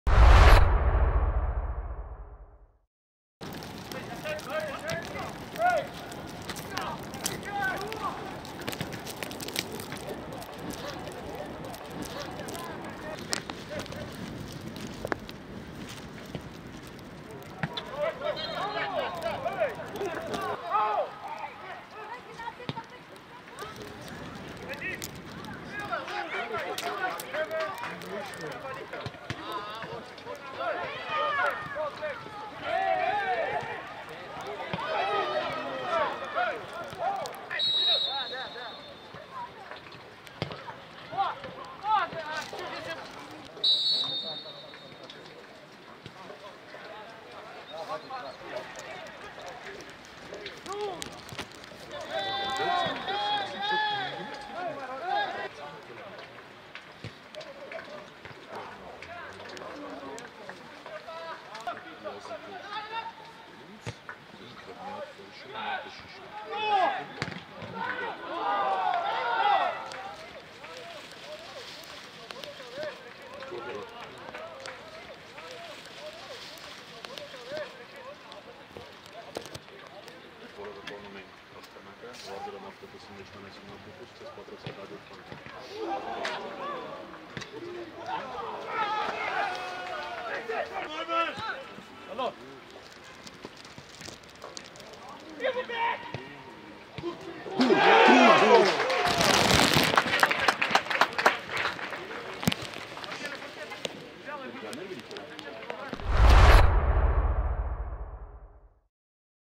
Friendly. Urartu FC - Ararat-Armenia FC 1-1. Full Highlights
During the international break, Urartu FC hold a friendly fixture with Ararat-Armenia FC at home. The match ended in a 1-1 draw. Armen Hovhannisyan scored the goal for Ararat-Armenia FC, and Karen Melkonyan scored the return goal for Urartu FC. Here you can watch the highlights of that match.